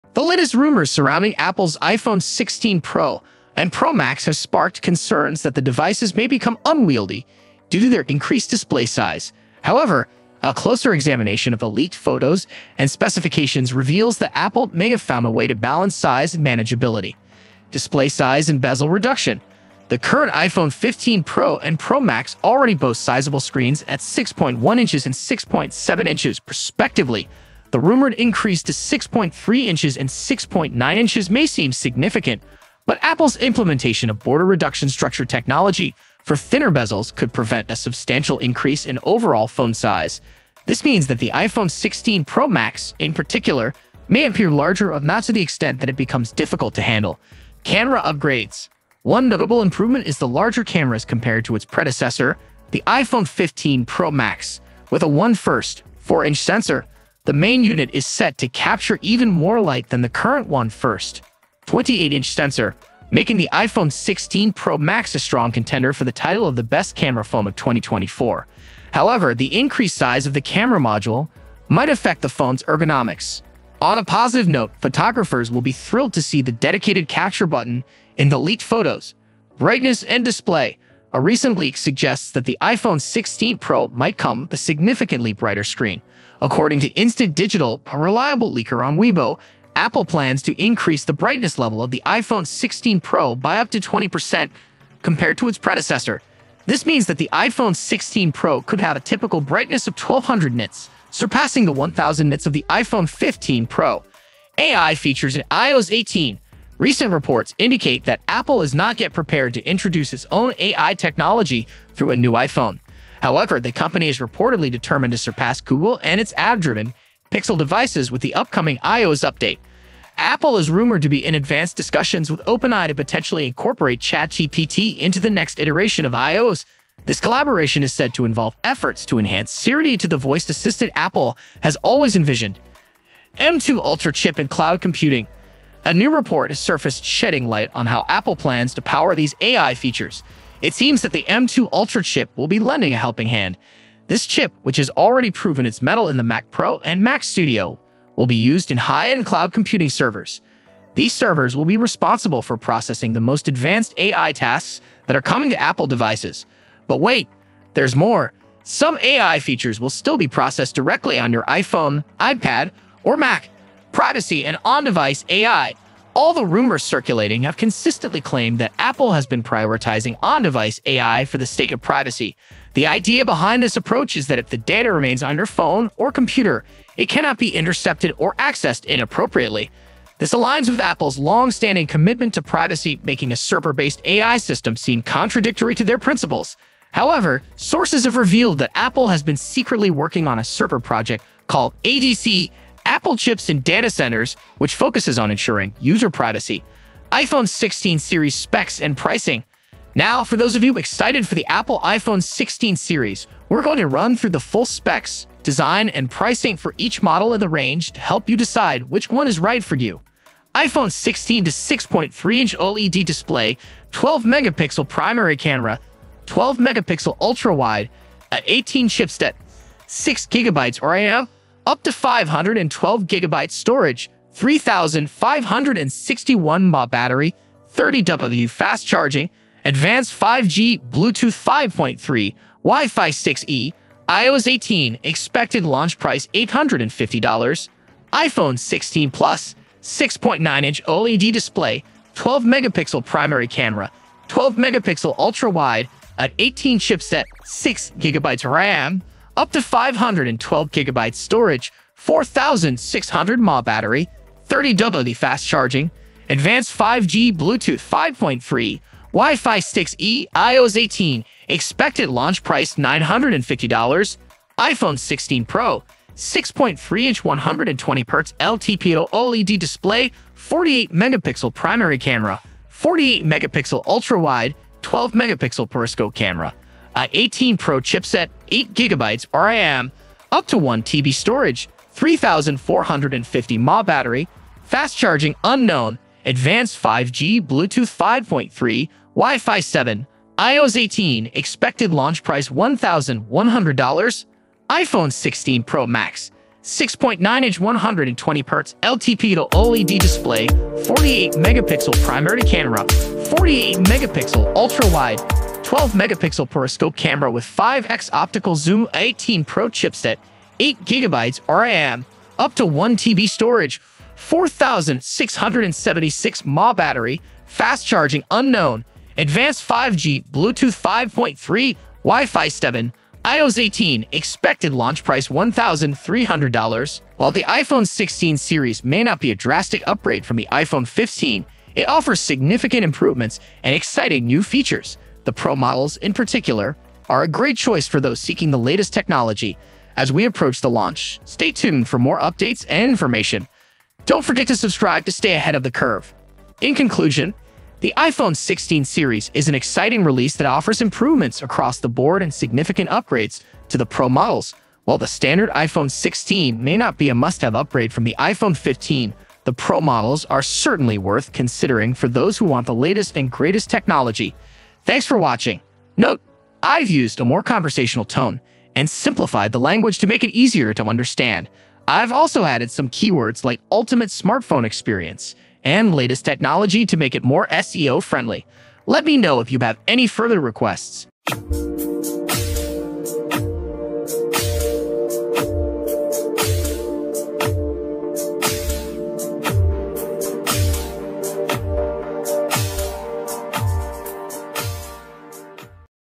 iPhone 16 Pro Max - MORE 4 UPDATES 🔥🔥
iPhone 16 Pro Max - MORE 4  UPDATES 🔥🔥 - In this video, we're diving into the top 4 updates that will take your smartphone experience to the next level. From a powerful A18 Pro chipset to an impressive 48-megapixel primary camera, we're covering it all. Plus, find out what's new with the 6.9-inch 120Hz LTPO OLED display and how the advanced 5G capabilities will change the game. Don't miss out on this in-depth look at the iPhone 16 Pro Max - watch now and stay ahead of the curve!